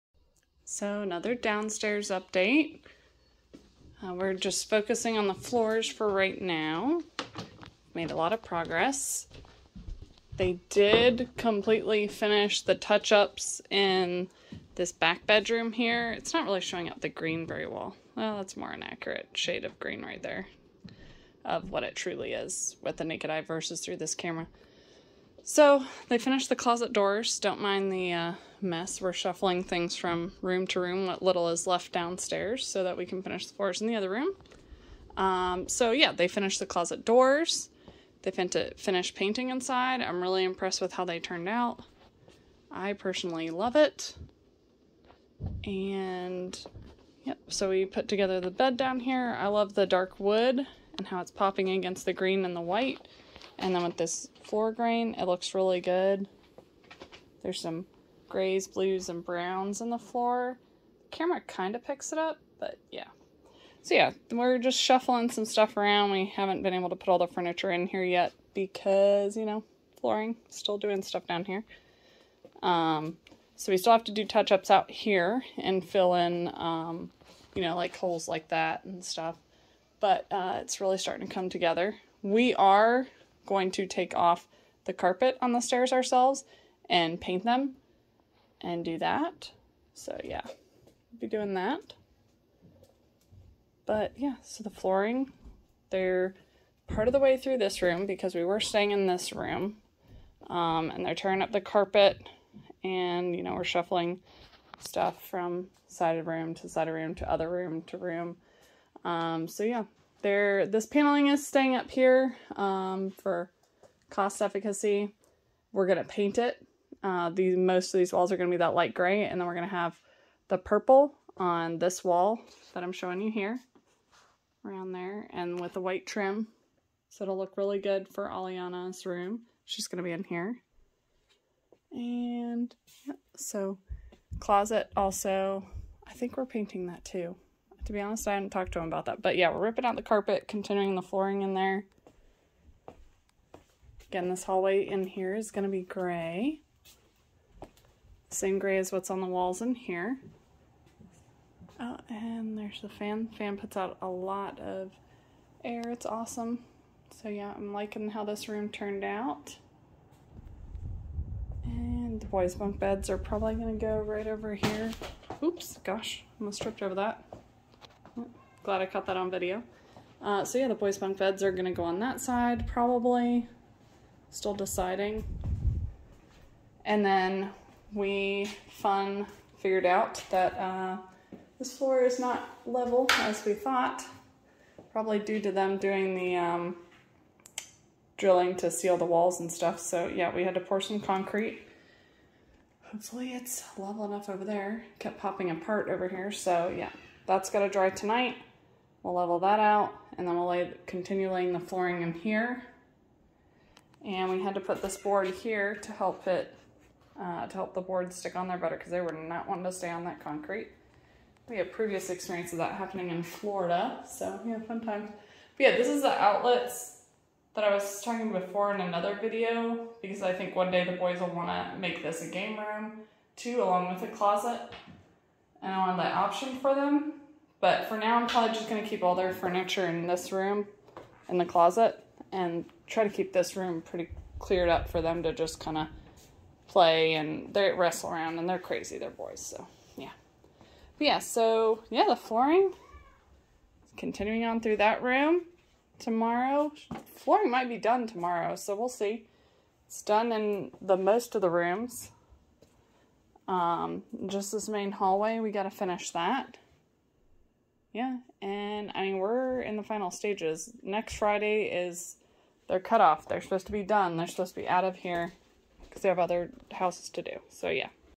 It's looking like a house again!
Flooring is about halfway complete. A few hiccups showed up like they always do, but we have made so much progress!